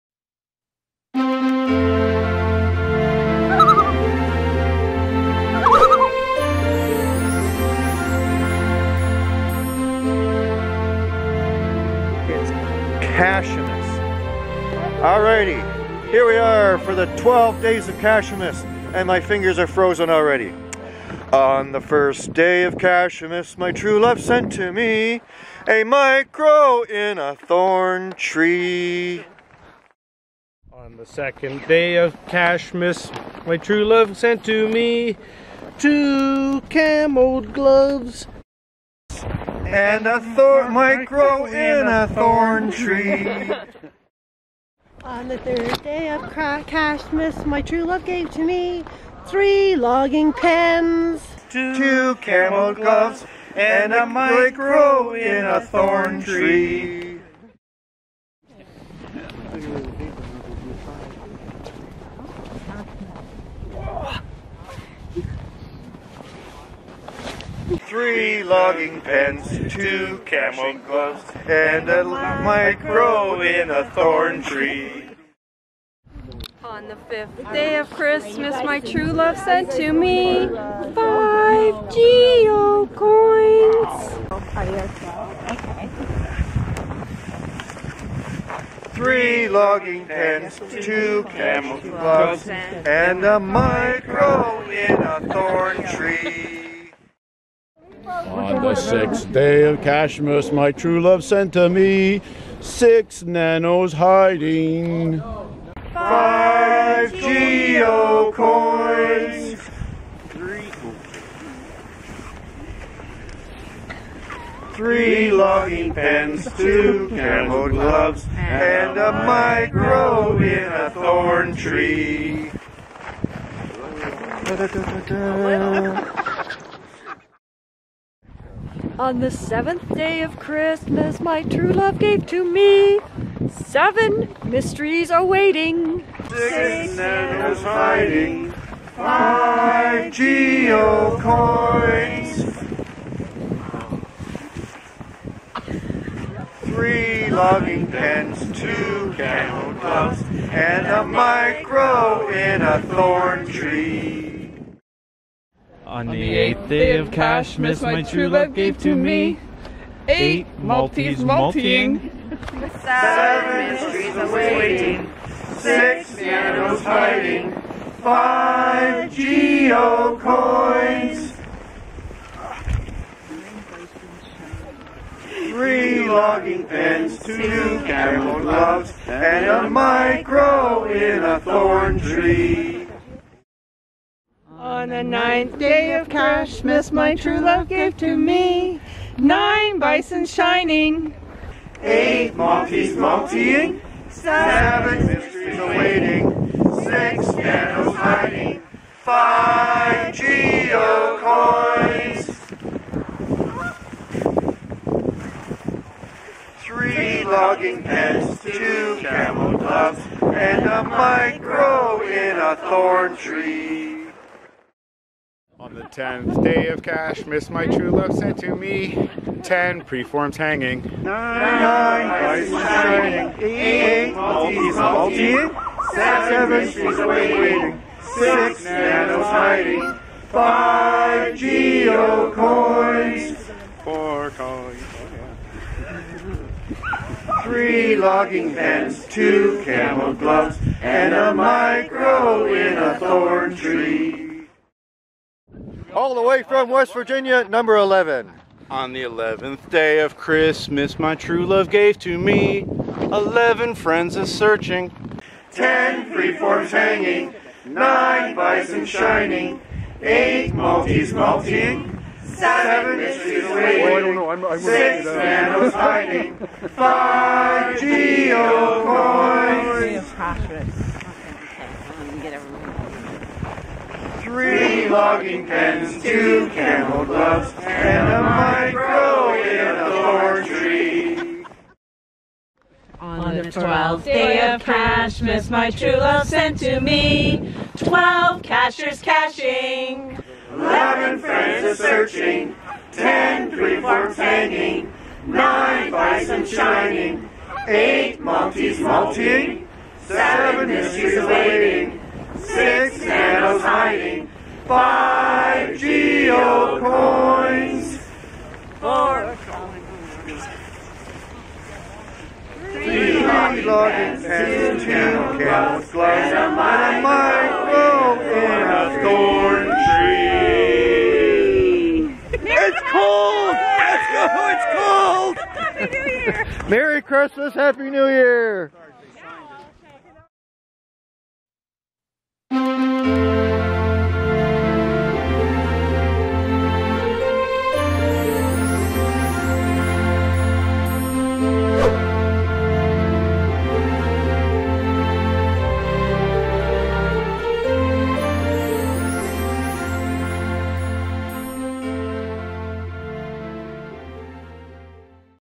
The 12 Days of Cachemas from Cache Canada to You
A Christmas message from Cache Canada to all of You. Thank you, Merry Christmas and a Happy Caching New Year.